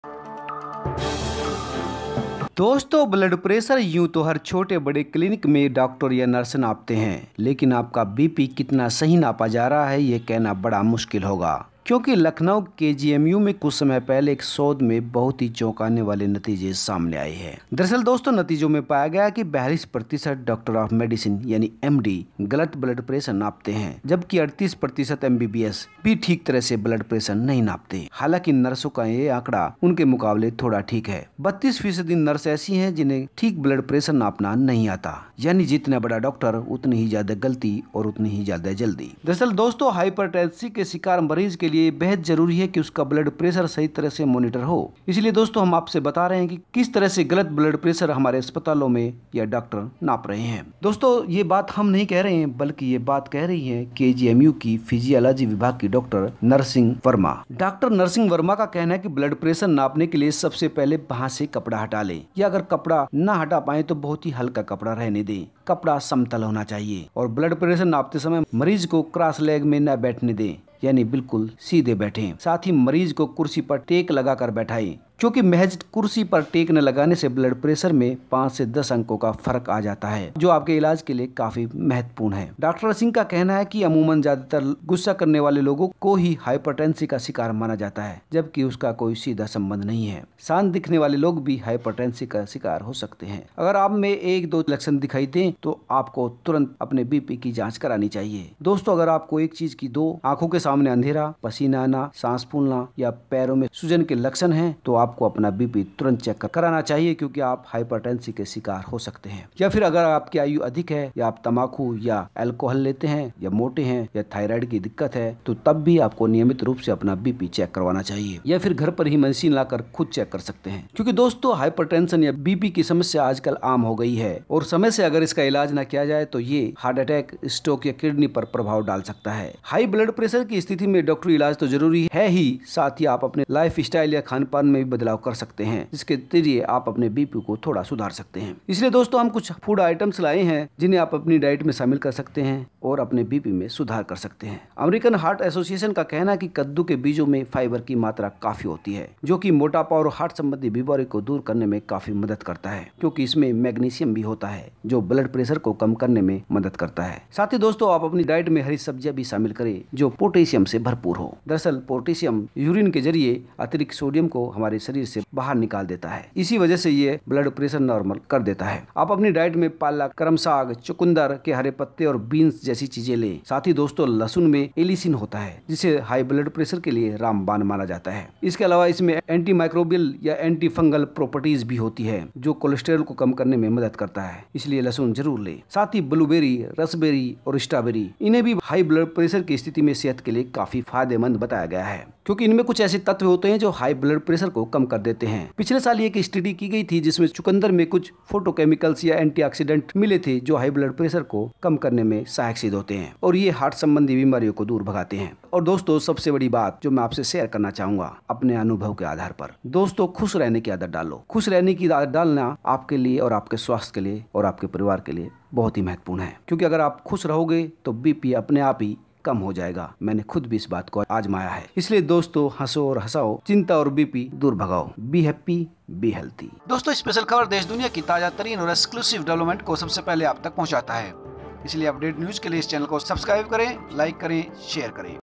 KNOW BLOOD PRESSURE MEASURE RIGHT TECHNIQUES ब्लड प्रेशर नापने और ठीक करने की तकनीक
ब्लड प्रेशर यूं तो हर छोटे-बड़े क्लिनिक डॉक्टर या नर्स नापती
है, लेकिन आपका बीपी सही नापा या गलत ये कहना जरा मुश्किल होता है। लखनऊ
केजीएमयू में कुछ समय पहले हुए एक शोध में बहुत ही चौंकाने वाले नतीजे
आय़े हैं. नतीजों में पाया गया कि 42 प्रतिशत डॉक्टर ऑफ मेडिसिन
(एमडी) गलत ब्लड प्रेशर नापते हैं जबकि 38 प्रतिशत एमबीबीएस को भी यह सही
से नापना नहीं आता। हालांकि, नर्सों में यह आंकड़ा कम है, महज 32 फीसदी
नर्स ऐसी है जिन्हें सही ब्लड प्रेशर नापना नहीं आता।  यानि इतना बड़ा
डॉक्टर उनती ही ज्यादा जल्दी और गलती करता है...
ब्लड प्रेशर की सही मॉनिटरिंग होना है जरूरी
हाइपरटेंसी का शिकार मरीजों के लिए बेहद जरूरी है कि उनके ब्लड प्रेशर की
सही मॉनिटरिंग हो। दोस्तों  यह बात हम नहीं कह रहे हैं यह बात केजीएमयू
के फीजियॉलजी विभाग के डॉक्टर नरसिंह वर्मा ने हाइपरटेंशन पर आयोजित एक
कार्यक्रम के दौरान कही। हाईपरटेंसी सप्ताह की शुरुआत पर रविवार को
हुसैनगंज के एक निजी होटल में ऑमरान अकैडमी की ओर से एक कार्यक्रम का
आयोजन हुआ। इसमें डॉक्टर्स और नर्सों को बताया गया कि कैसे मरीजों का
ब्लड प्रेशर नापा जाए।
चलिए तो दोस्तों हम आपको बताते है कि बीपी चेक कराने जा रहे हैं तो किन
बातों का रखें ख्याल
डॉक्टर नरसिंह ने बताया कि ब्लड प्रेशर नापने के लिए सबसे पहले बांह से
कपड़े को हटा दें या हल्का कपड़ा रखें। कपड़ा समतल होना चाहिए। ब्लड
प्रेशर नापते समय मरीज क्रॉस लेग न बैठे, साथ ही मरीज को कुर्सी पर टेक
लगाकर बैठाएं। महज कुर्सी पर टेक न लगाने से ब्लड प्रेशर में 5-10 अंकों
का फर्क आ जाता है जो अपने आप में इलाज करने के लिहाज से काफी ज्यादा
होता है।
डॉक्टर सिंह ने बताया कि अमूमन ज्यादा गुस्सा करने वाले लोगों को
हाइपरटेंसी का शिकार माना जाता है जबकि इससे उसका कोई संबंध नहीं। शांत
दिखने वाले भी हाइपरटेंसी का शिकार हो सकते हैं। अगर आप में एक चीज दो
दिखना, आंखों के सामने अंधेरा छाना, पसीना आना, सांस फूलना, पैरों में
सूजन जैसे लक्षण हैं तो आप हाइपरटेंसी के शिकार हो सकते हैं। अगर आपकी
आयु अधिक है, तम्बाकू या एल्काहॉलिक हैं, मोटापा है या थाइरॉयड की दिक्कत
है तो नियमित ब्लड प्रेशर की जांच करवाएं। घर पर ब्लड प्रेशर नापने की
मशीन रखें।
दोस्तों ब्लड प्रेशर की समस्या आजकल काफी आम हो गई है। ज्यादातर लोग
हाइपरटेंशन यानी हाई बीपी से परेशान हैं। अगर इसका समय पर इलाज न किया
जाए तो हार्ट अटैक, स्ट्रोक और किडनी पर प्रभाव पड़ सकता है। हाई ब्लड
प्रेशर की स्थिति में डॉक्टरी इलाज तो जरूरी है ही साथ ही लाइफस्टाइल और
खान-पान में भी बदलाव करने की जरूरत है। दोस्तों हम आपको कुछ फूड आइटम्स
बता रहें हैं, जिन्हें आप अपने डायट में शामिल कर सकते हैं:... अमेरिकन
हार्ट असोसिएशन के मुताबिक, कद्दू के बीजों में फाइबर की मात्रा काफी
अधिक होती है जोकि मोटापा औरहार्ट संबंधी बीमारियों को दूर करने में मदद
करता है। इसके अलावा इसमें मैग्निशियम भी  होता है जो ब्लड प्रेशर को कम
करने में मदद करता है।साथ ही दोस्तों  अपने डायट में ऐसी हरी सब्जियां
शामिल करें जो पोटेशियम से भरपूर हों। दरअसल पोटेशियम यूरिन से अतिरिक्त
सोडियम को बाहर निकालने में किडनी की मदद करता है। इसकी वजह से ब्लड
प्रेशर नॉर्मल हो जाता है। आप अपनी डायट में पालक, करमसाग, चुकंदर के हरे
पत्ते और बीन्स जैसी चीजें लें।लहसुन में ऐलिसिन होता है जिसे हाई ब्लड
प्रेशर के लिए 'रामबाण' माना जाता है। इसके अलावा इसमें ऐंटीमाइक्रोबियल
और ऐंटीफंगल प्रॉपर्टीज़ भी होती हैं जो कलेस्ट्रॉल को कम करने में मदद
करती हैं।ब्लूबेरी, रसबेरी और स्ट्रॉबेरी...इन्हें भी हाई ब्लड प्रेशर की
स्थिति में सेहत के लिए काफी फायदेमंद बताया जाता है। इनमें में कुछ ऐसे
तत्व होते हैं जोकि ब्लड प्रेशर को कम करते हैं।पिछले साल की गई एक स्टडी
में बताया गया था सि चुकंदर में कुछ फोटोकैमिकल और ऐंटी-ऑक्सिडेंट्स होते
हैं जोकि हाई ब्लड प्रेशर से लड़ने में सक्षम होते हैं। साथ ही ये हार्ट
संबंधी बीमारियों को भी दूर करने में मदद करते हैं।..और सबसे बड़ी बात
दोस्तों  चिंता छोड़ों... खुश रहने की आदत डालों.. आप खुश रहने की आदत
डालोगे तो बीपी अपने आप कम हो जाता है...मैने खुद इस बात को आजमाया
है. इसलिए दोस्तों हंसो और हंसाओं. चिंता और बीपी दूर भगाओं...
42% MD 38% MBBS Doctor Charge wrong blood pressure '
KNOW BLOOD PRESSURE TECHNIQUES
Friends, blood pressure does not mean every small clinic doctor or
nurse, but it is difficult to say that your BP is right or wrong. In
Lucknow KGMU, a research done some time ago has very shocking results
. The findings found that 42% of doctors of medicine (M.D.) impose
false blood pressure, while 38% of MBBS does not even accurately
measure it. Comes. However, this figure is low in nurses, only 32
percent of those who are nurses do not have the right blood pressure
to measure. That is, such a big doctor makes them very quickly and
mistake ...
Need to have accurate blood pressure monitoring
It is very important for patients suffering from hypertension to have
proper monitoring of their blood pressure. Friends, this is not what
we are saying. This is the talk of KGMU's physiology department doctor
KGMU's physiology department  during a program organized on hypertension. A
program was organized on